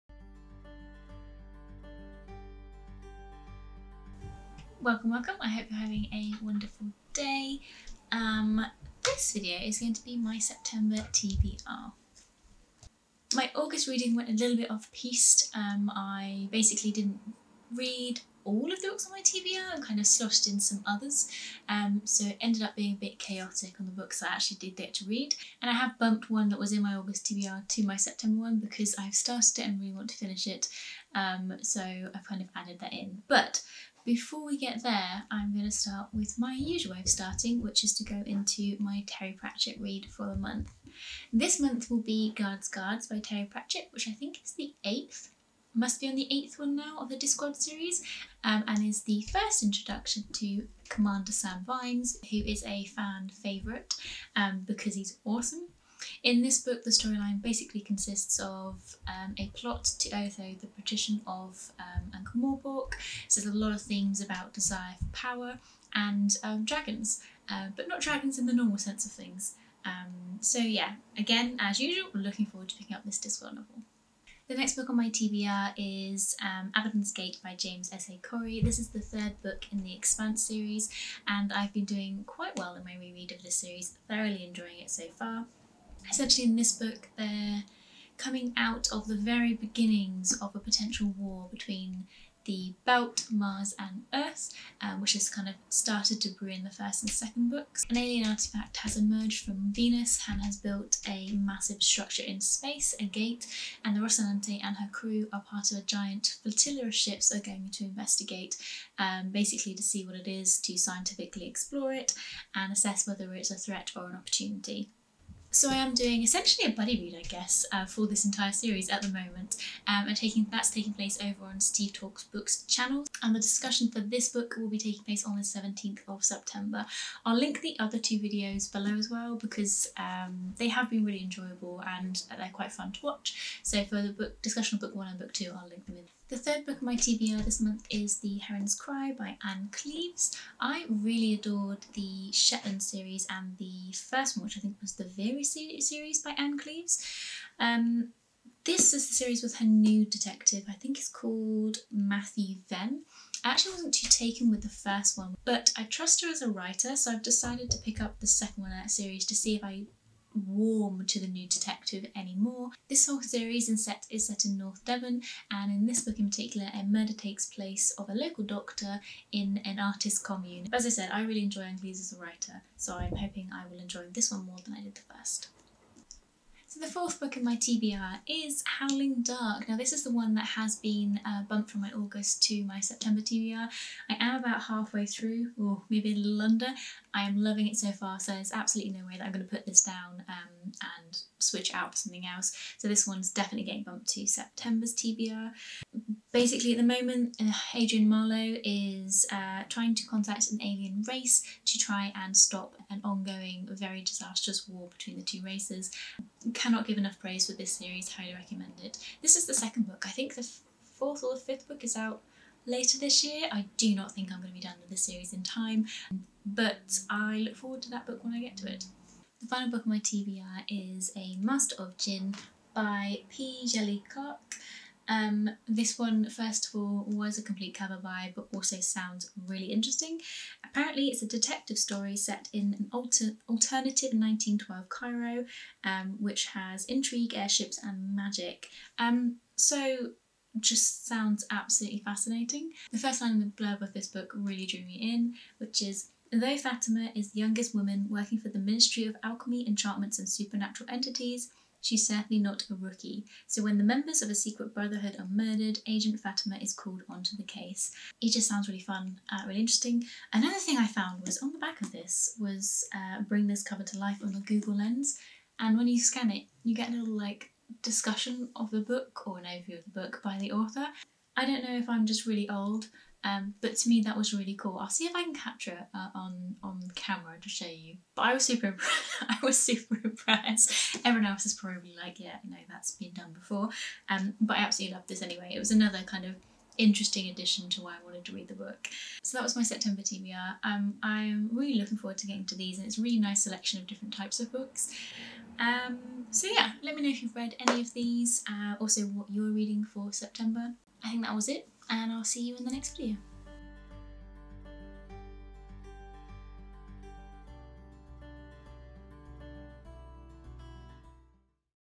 September TBR!
Taking a look at what I will be reading (or trying to read in the month of September). Now I've switched slightly I feel like I'm actually getting through some of the series yay!